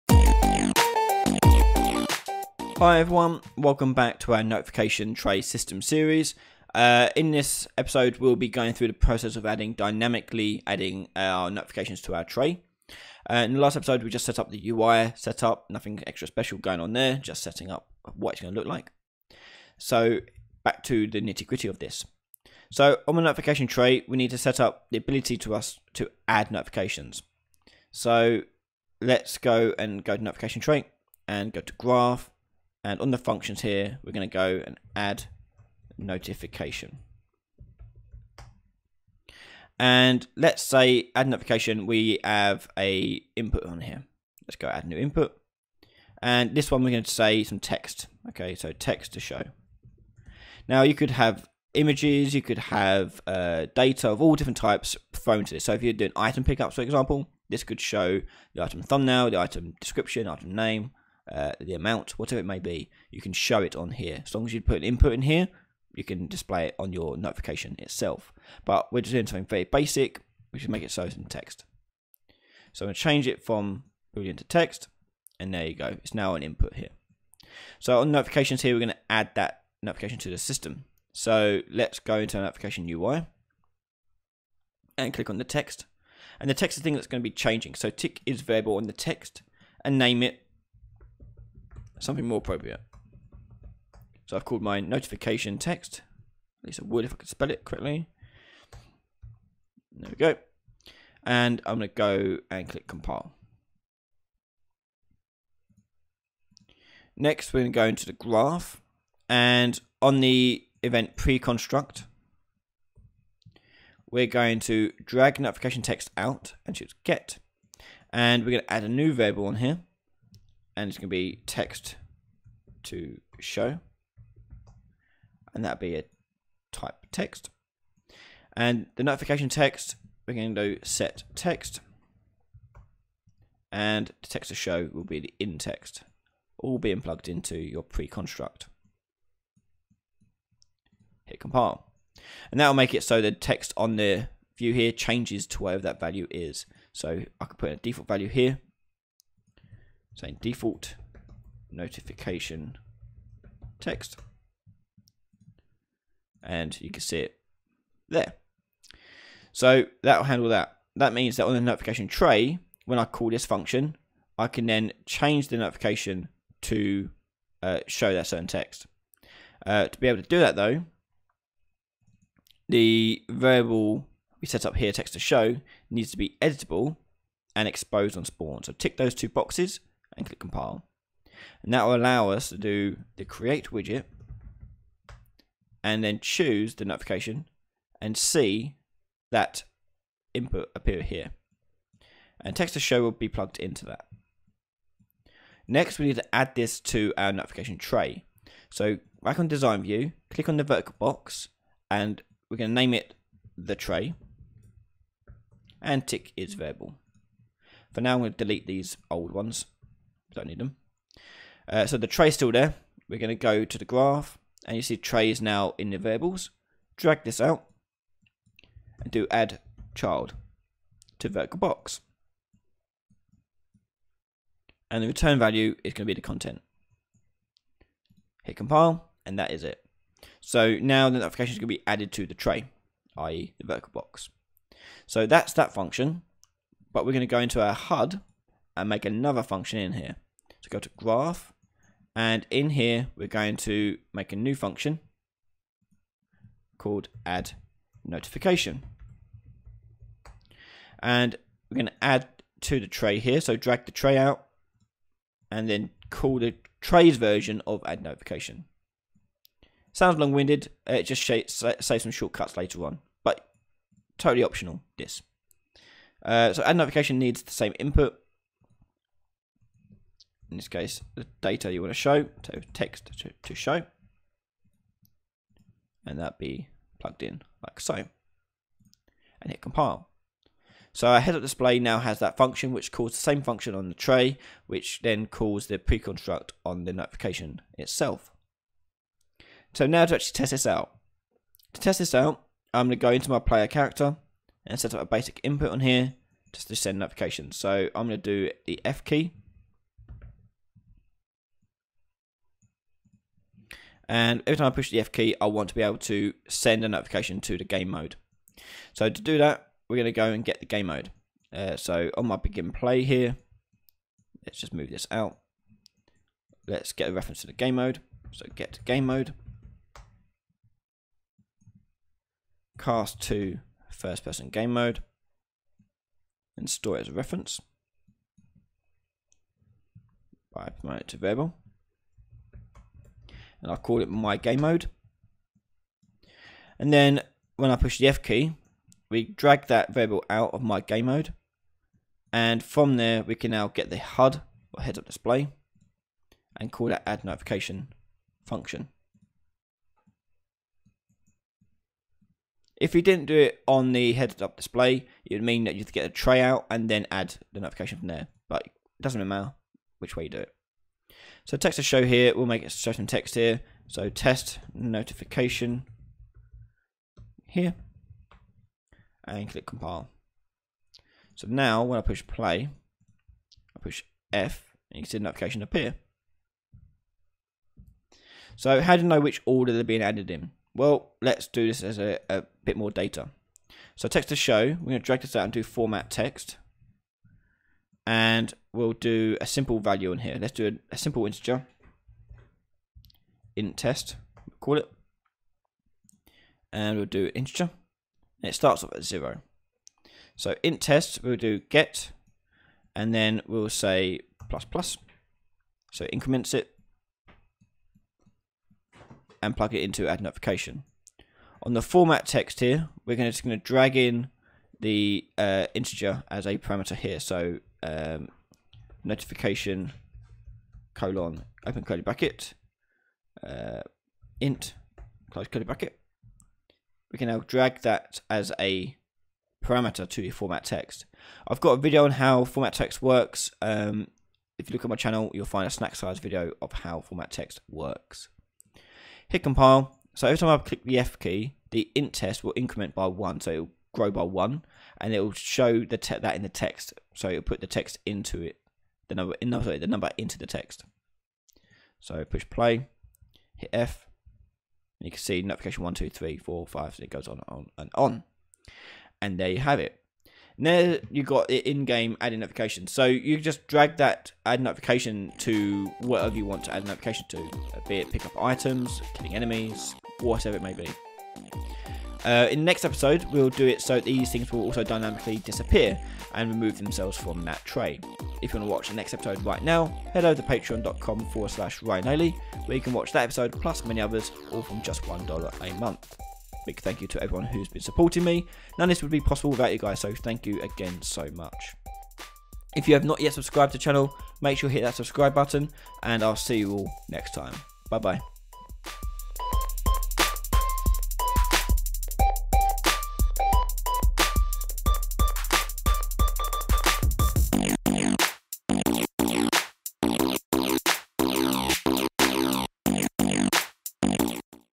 Unreal Engine 4 Tutorial - Notifications Part 2: Adding
In this series we implement a notification system to display notices to the player in a vertical list on the side of the screen.

Part 2 we go through how to add notifications to the tray in-game.